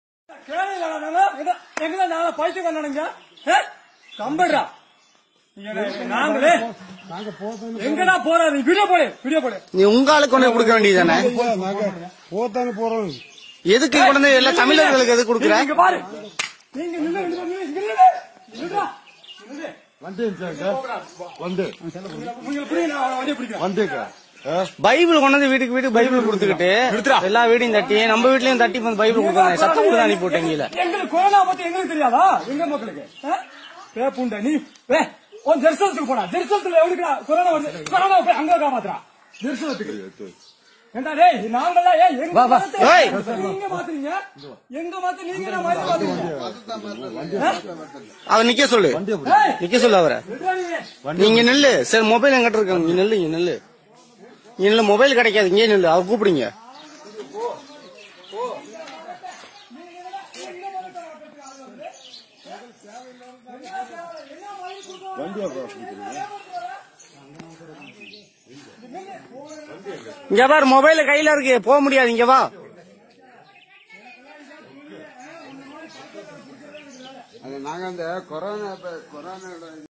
Pr Johnson from Salem, Tamil Nadu was attacked by Fanatics Church Pray for the Persecutors
Pastor Johnson from Salem district in Tamil Nadu was attacked by Fanatics. Church Pray for the Persecutors